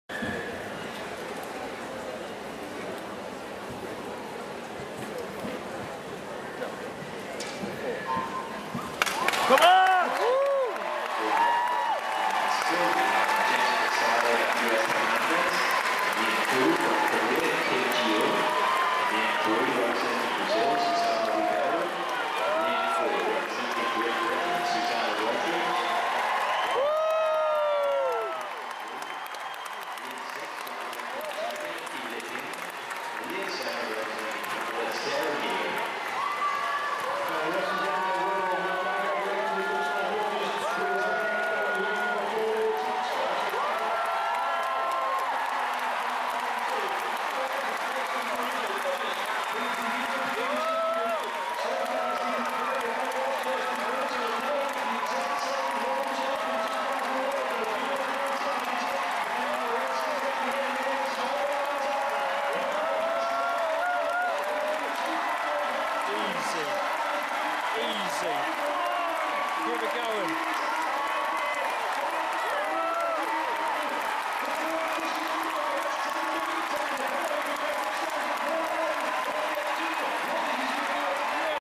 London 2012 Paralympics: Swimming - Women's 100m Freestyle S7 (Heat 1)
Filmed at the Aquatics Centre on Monday 3rd September, featuring Susanna Rodgers of Team GB.